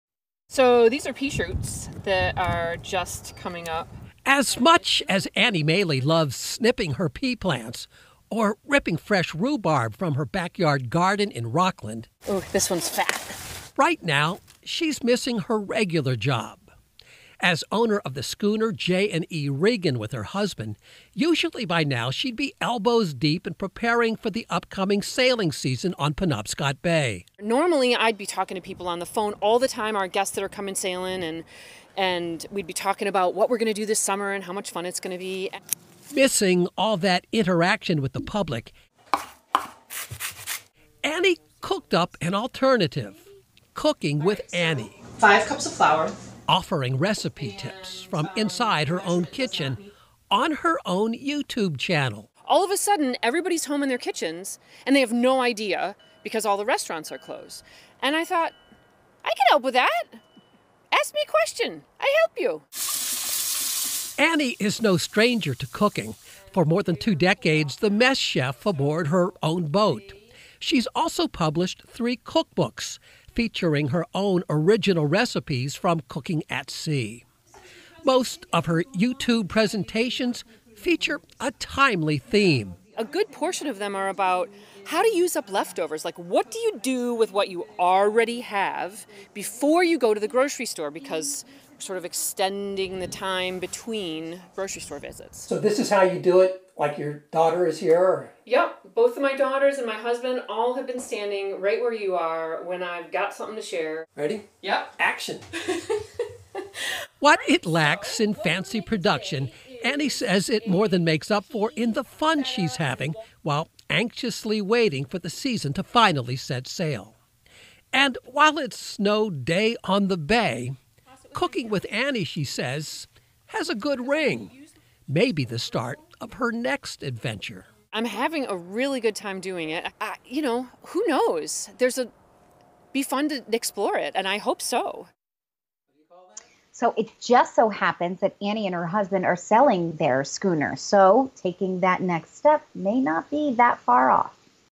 'Cooking with Annie': Maine woman launches YouTube channel to share recipes
A lot of Mainers are spending more time in the kitchen due to the coronavirus outbreak, but a Rockland woman took it further and launched a YouTube channel to share her recipes.